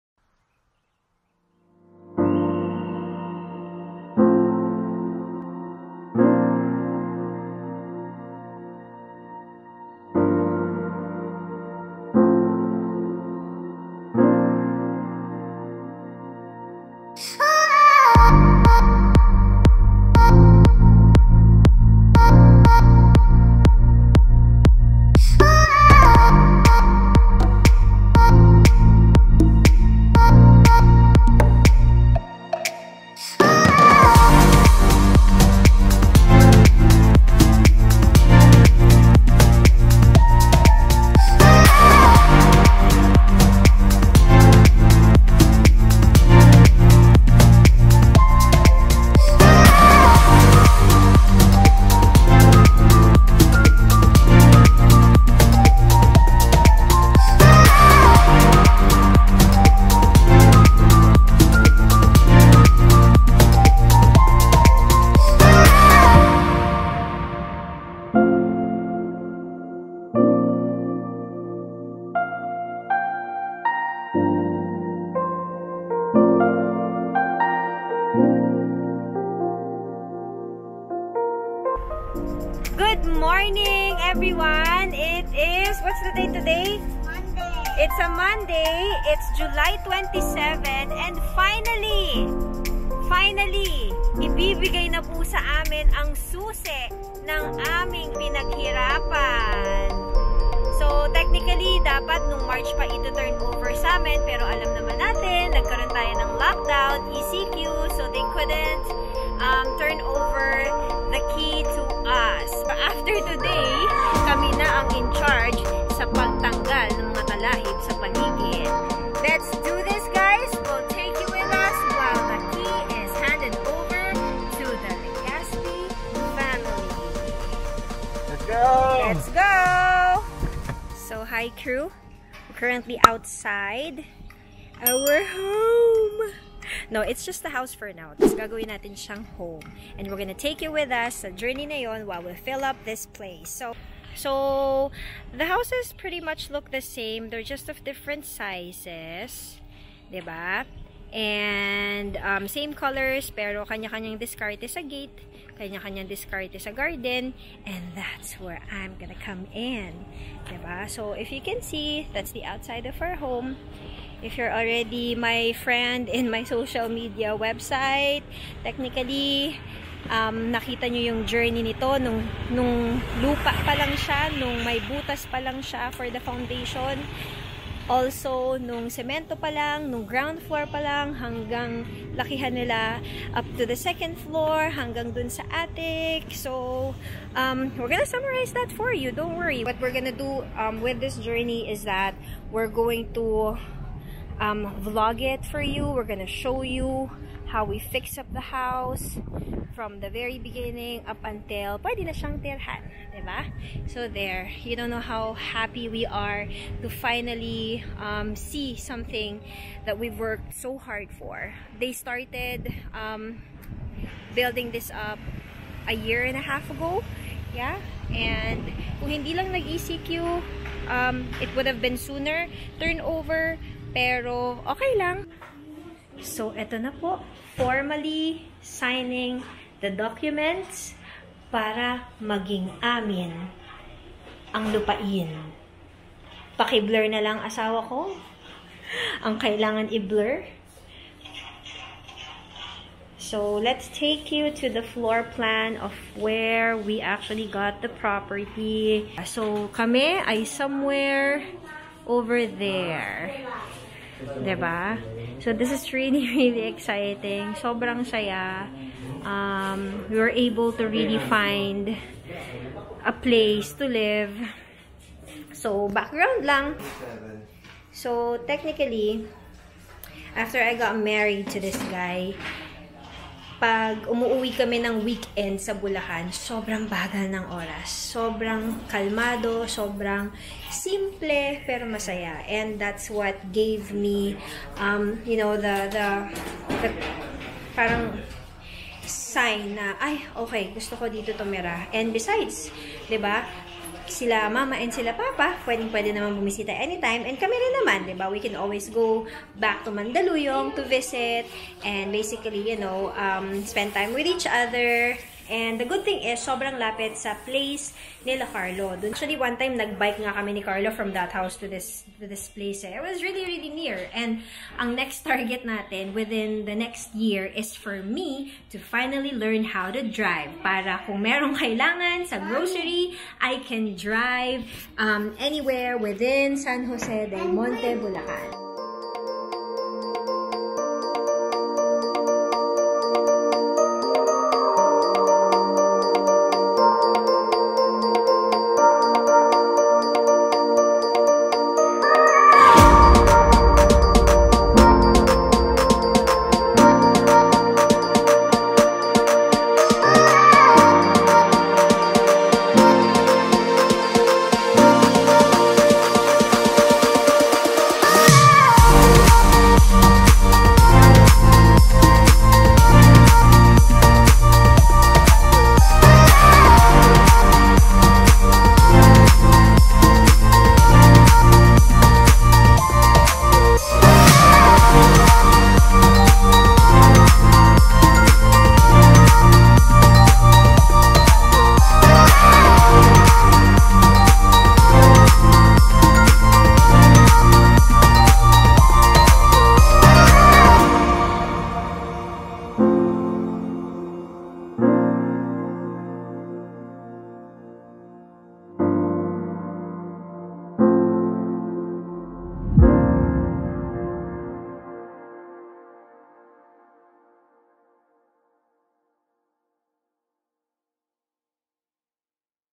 House Turnover | Our Own Home | Hard Work Paid Off
We've finally got a place of our own!
All the long nights, busy days, lack of sleep in years, everything has paid off!

This is OURS.🏡